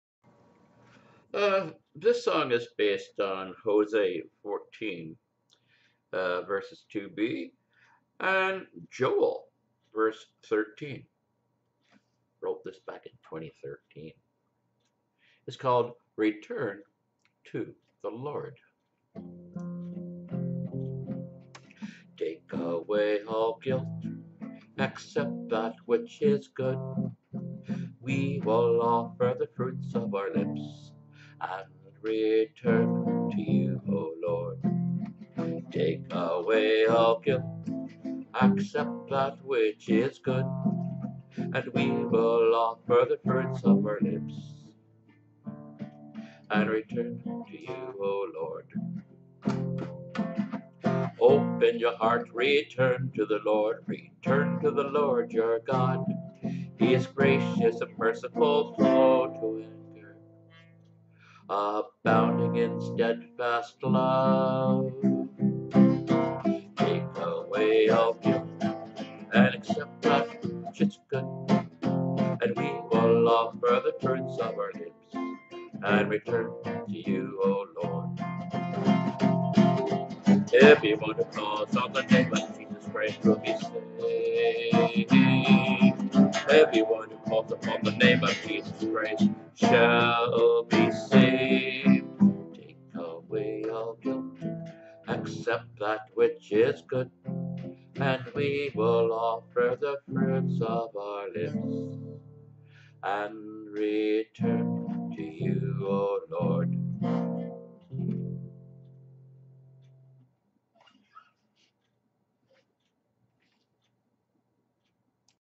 RETURN TO THE LORD
A prayer for forgiveness, based on Hosea 14 verse 2b and Joel 2 verse 13